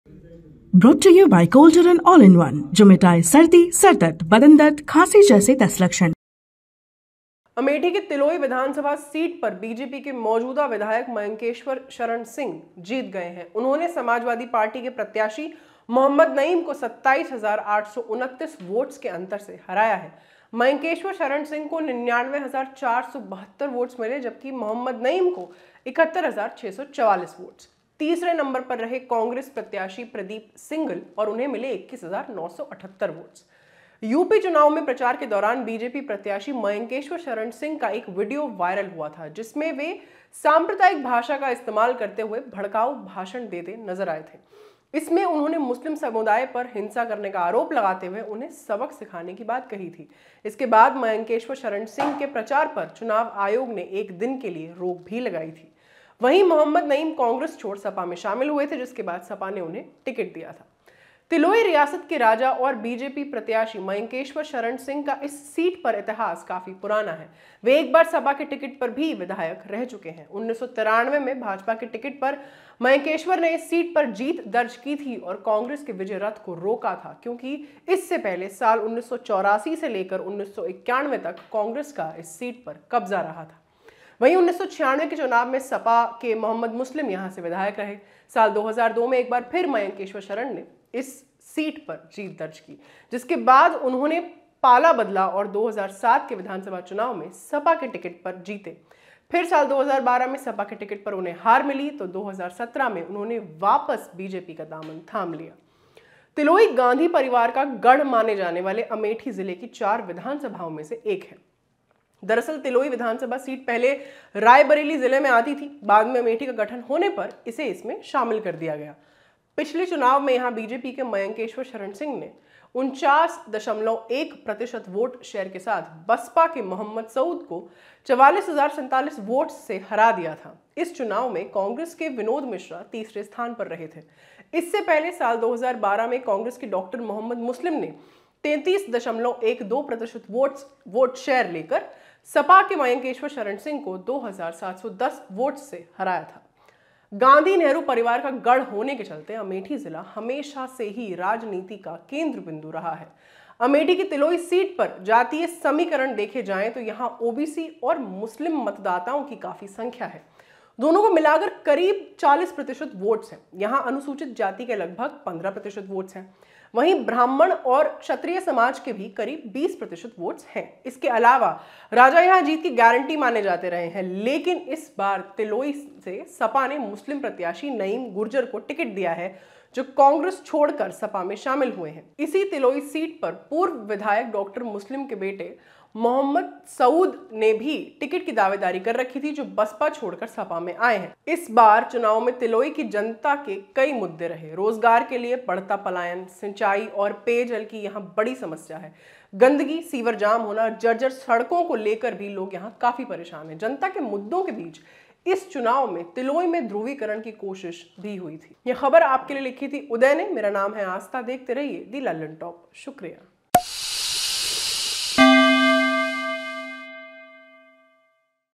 Muslims पर कॉमेंट करने वाले BJP Candidate Mayankeshwar जीते या हारे? UP Election Result
Mayankeshwar Sharan Singh has won from Tiloi constituency. Tiloi is amongst the 4 Vidhan Sabhas that fall under Amethi district. Mayankeshwar was in news in the past for giving hate speech against the Muslim community.